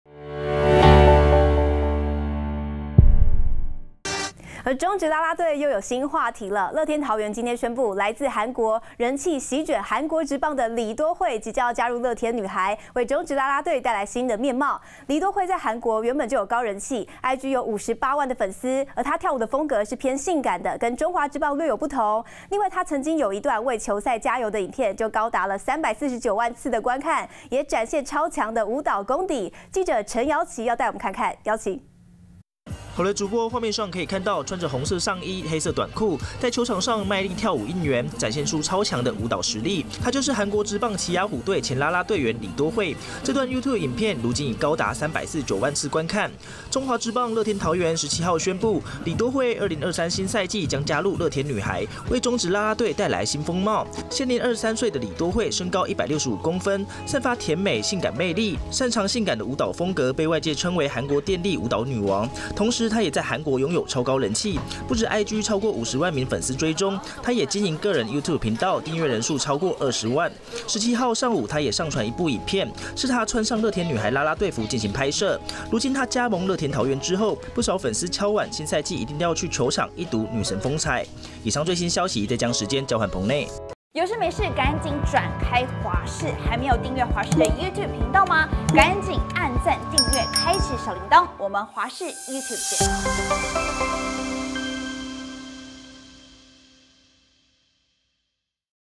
李多慧加入樂天啦啦隊 為中職帶來全新風貌｜華視新聞 20230317
今(17)日Rakuten Monkeys樂天桃猿今天宣布，來自韓國、人氣襲捲韓職的李多慧(Dahye Lee)將加入啦隊樂天女孩，為中職啦啦隊帶來新風貌。李多慧在韓國原本就高人氣，IG有58萬粉絲，跳舞的風格偏性感，和中職的略有不同，而她曾經一段為球賽加油的影片就高達349萬次觀看，展現超強的舞蹈底子，連線記者陳堯棋。